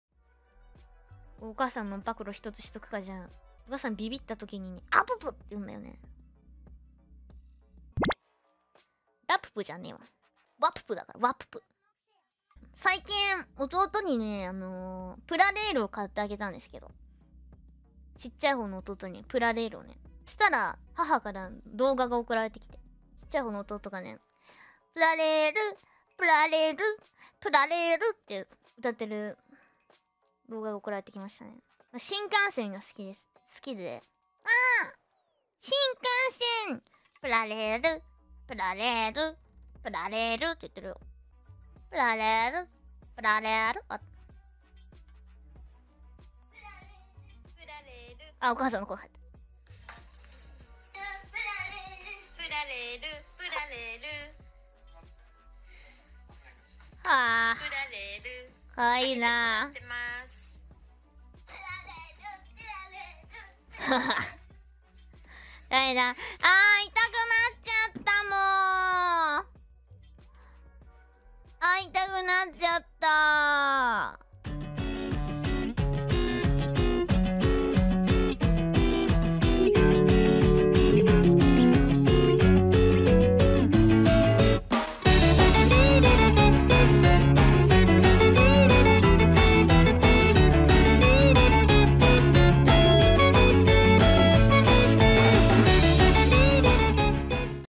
Laplus reveal suara ibunya dan adik laki-laki nya (Hololive/Sub Indo)
Kawaii~

kalau ada salah dalam penerjemahan bisa kasih tau di kolom komentar atau mau ngasih kritik saran boleh

魔王魂

୨୧┈┈┈┈┈┈┈┈┈┈┈┈┈┈┈┈┈┈୨୧
Source & Channel :